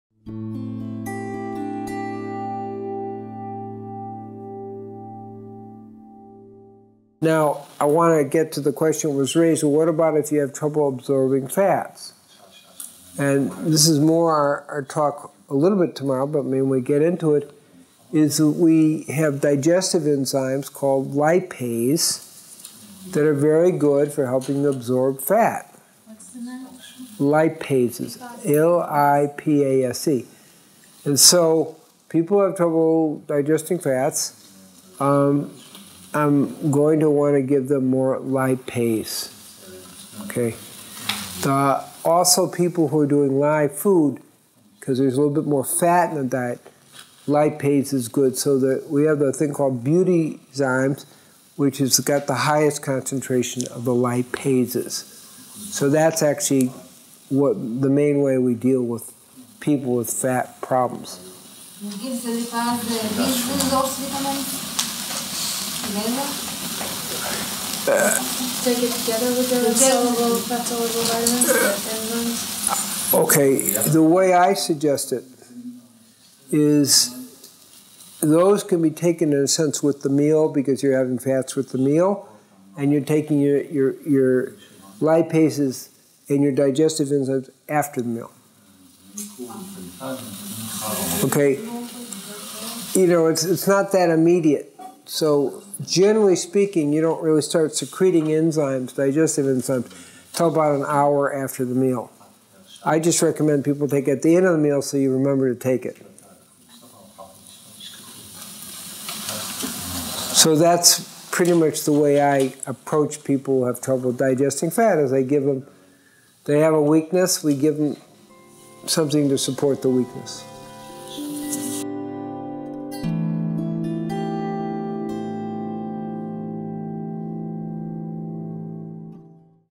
Q&A - What To Do If You Have Trouble Absorbing Fats? Lipase (Beauty Enzymes) | Gabriel Cousens MD
The MasterList Course
The Art of Healing with Herbs, Supplements & Superfoods

ML03 01B 14